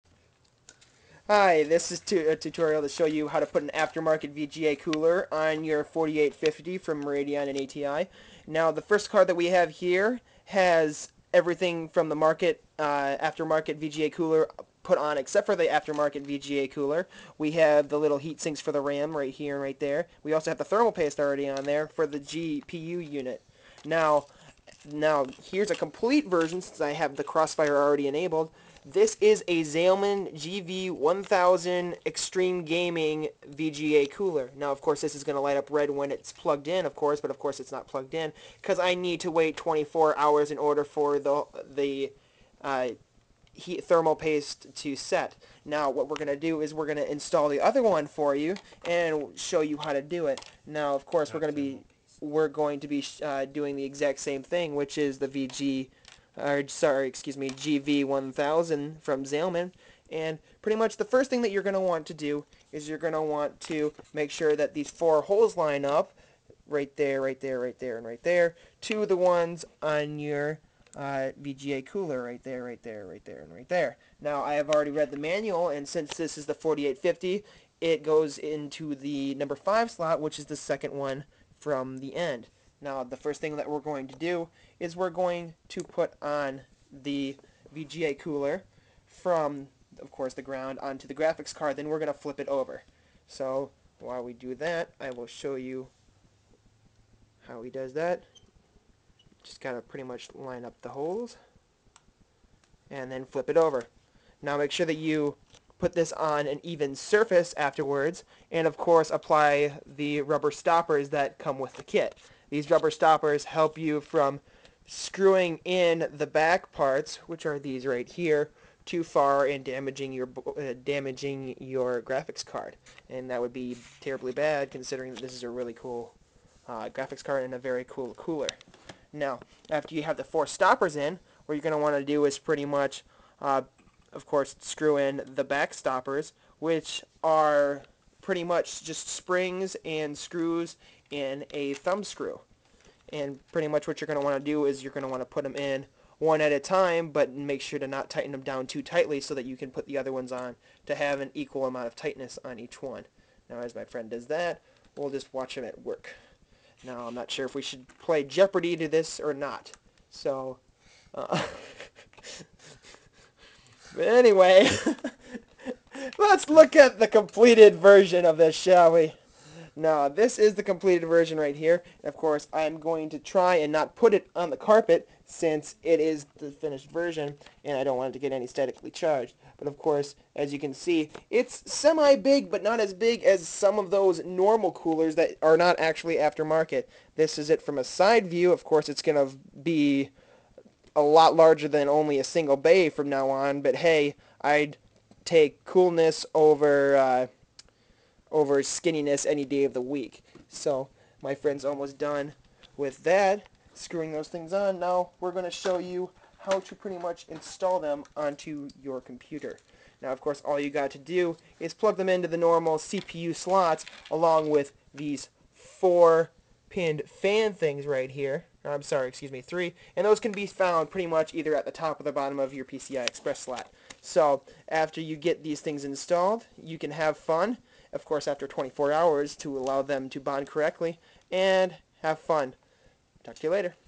radeon 4850 tutorial: Putting on an aftermarket vga cooler
This tutorial will show you the steps that I took in order to put together an aftermarket vga cooler. This tutorial deals in depth with the gv1000 vga cooler with Red LED lit fan from Zalman. We will show you how this easy install works as well as what to look for on your motherboard in order to finish a successful installation.